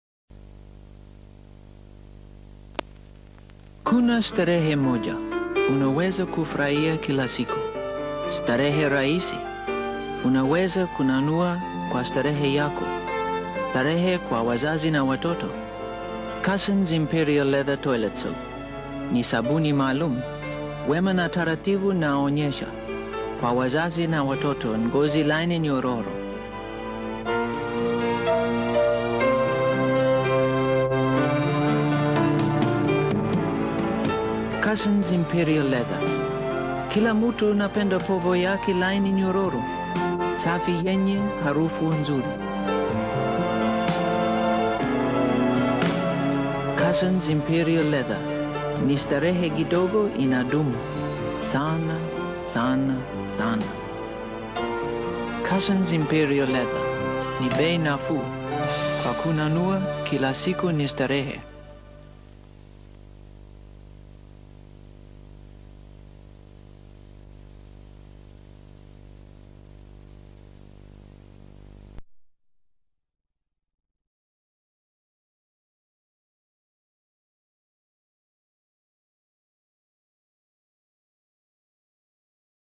CUSSONS IMPERIAL LEATHER BATHING SOAP ADVERT.
CUSSONS IMPERIAL LEATHER SOAP WAS THE BEST BATHING SOAP TO BUY IN THE 1980'S AND 1990'S. EVERYBODY LOVED THIS SOAP